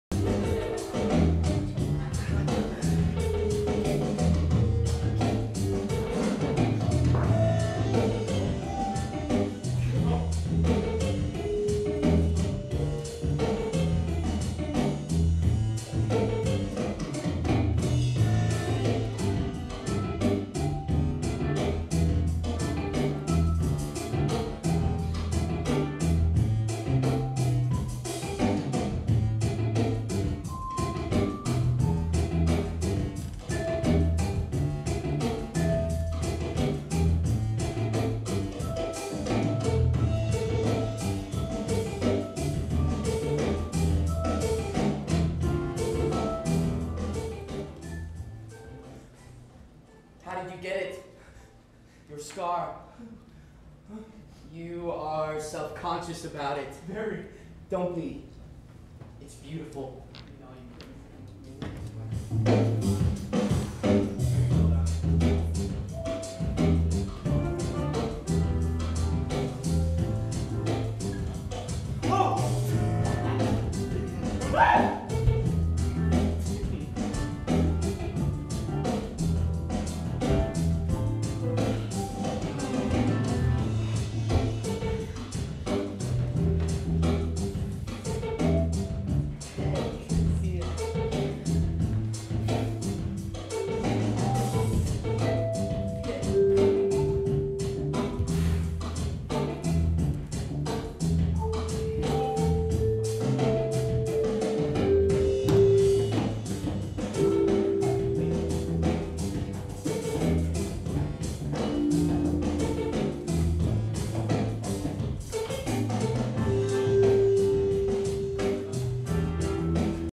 DORIAN at The House Theatre of Chicago
The House Theatre of Chicago's 2014 production of DORIAN, based on Oscar Wilde's novel 'The Picture of Dorian Gray'
By Tommy Rapley and Ben Lobpries
Directed and Choreographed by Tommy Rapley
Clip from Act 1, where Dorian meets Basil and Basil begins to paint Dorian's portrait. Patrick Andrews as Basil and Cole Simon as Dorian.